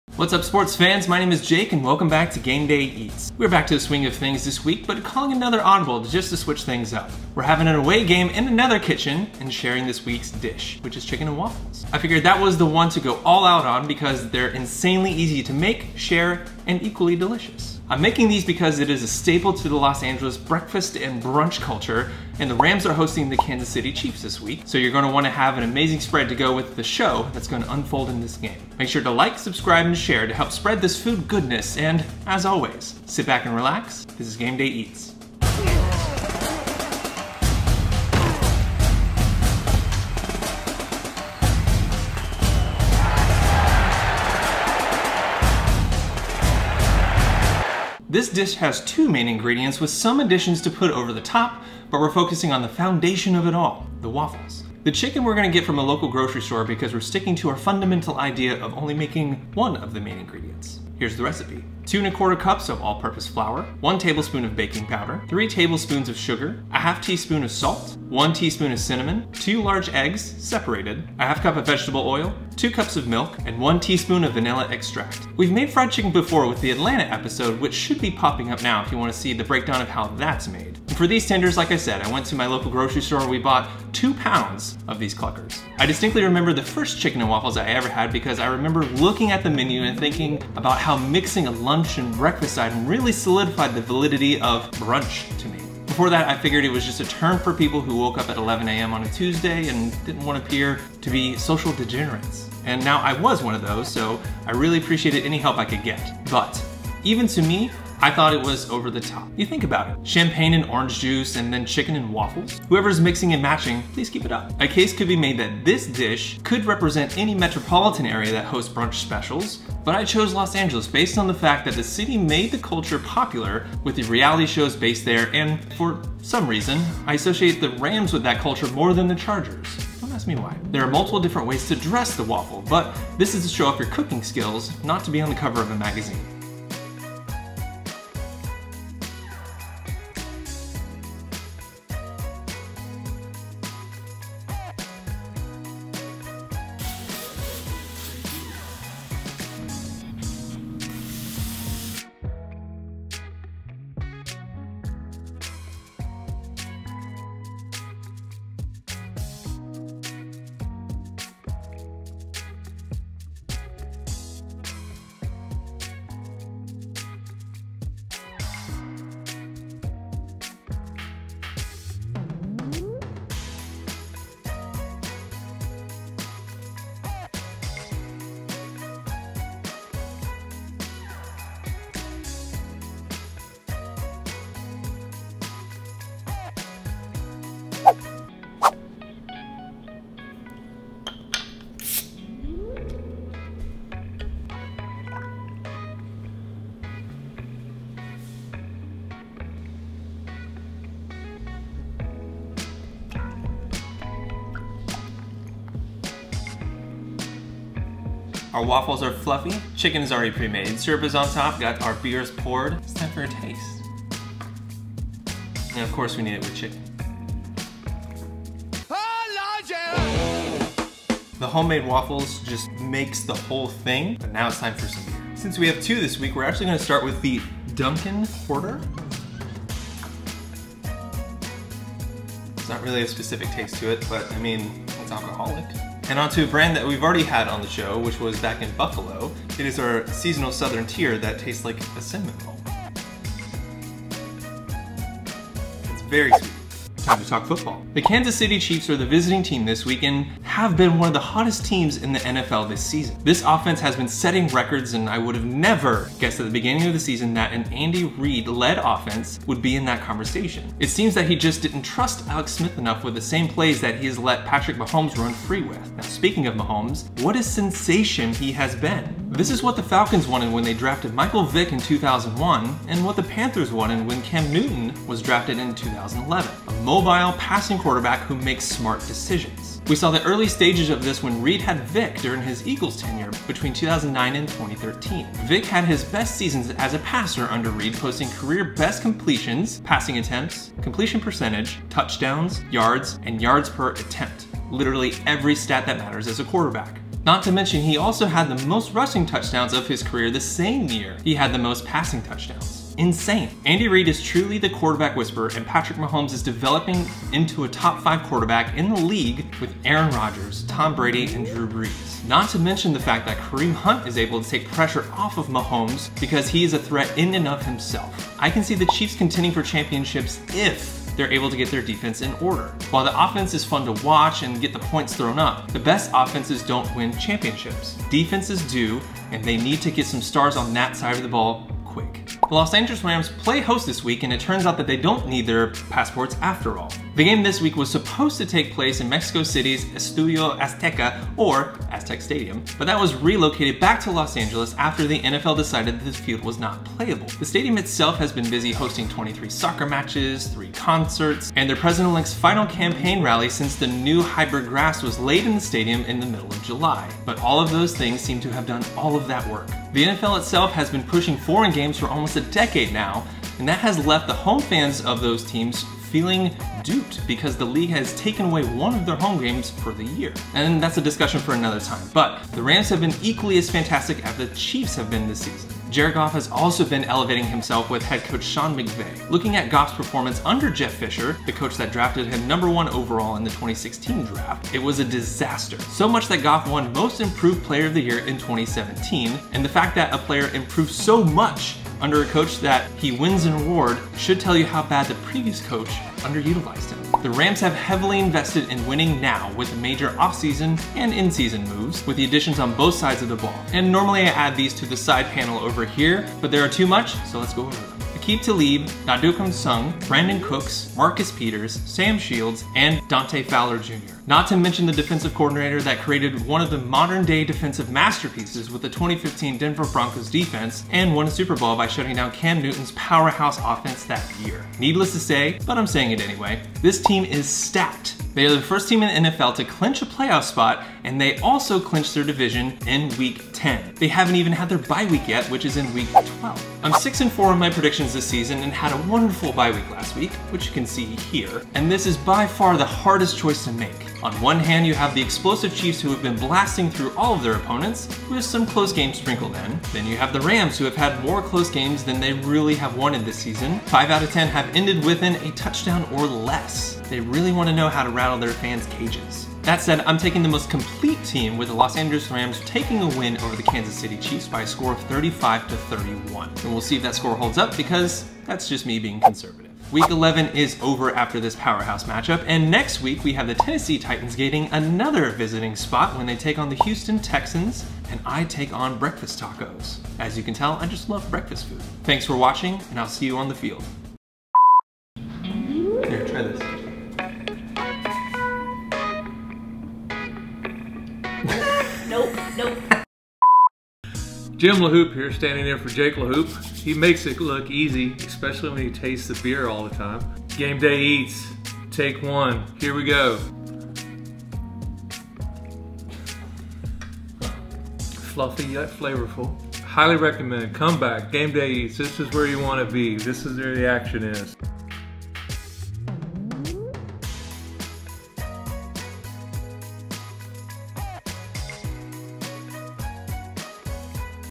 Did She Get a Bite of the Chicken and Waffles Before I Did??
This week Game Day Eats visits another kitchen to cook up some chicken and waffles because the Kansas City Chiefs are visiting the Los Angeles Rams in LA, instead of down in Mexico City. More on that in the show. If you only make ONE thing from this series it HAS to be this! Incredibly easy to make, as you'll see in the video. Enjoy.